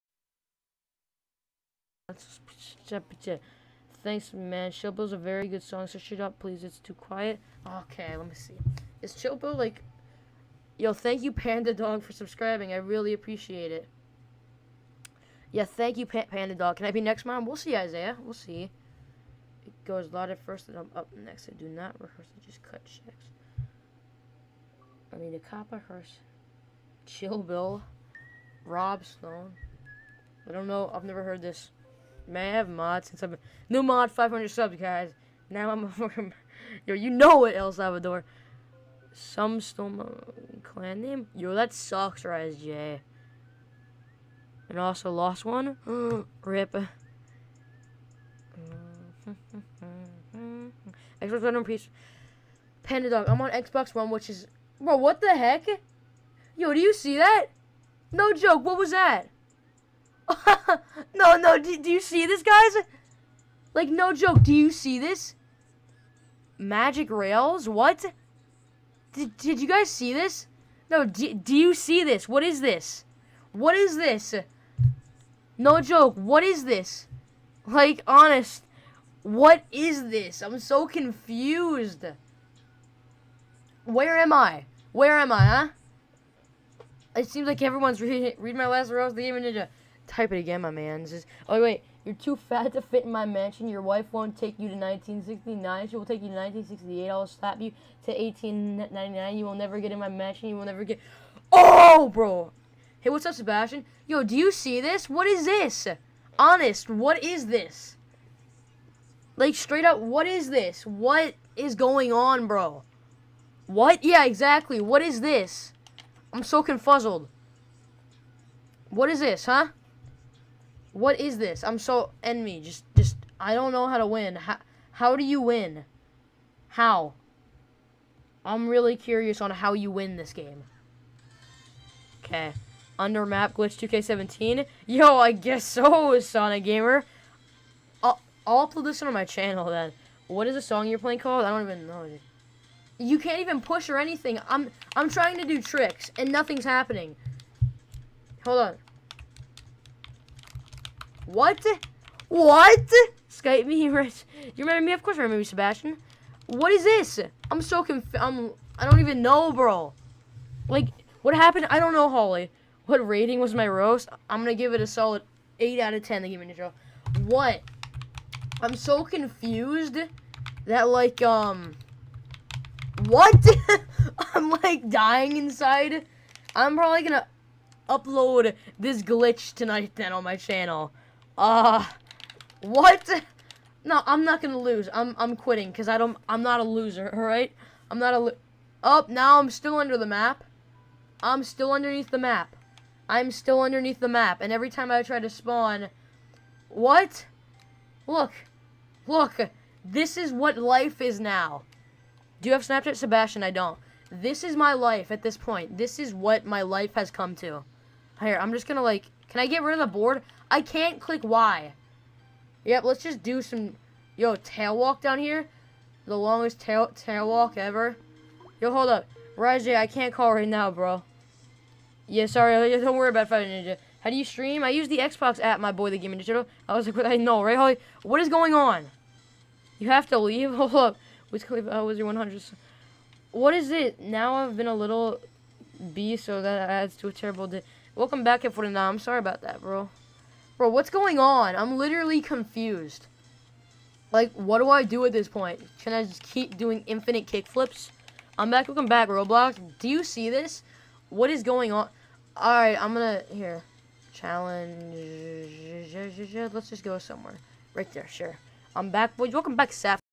UNDER THE MAP SESSION SPOT GLITCH!!
Shout out to Holly for hosting this game and getting this glitch! lol, but anyways there will be some more videos coming out either today or tomorrow!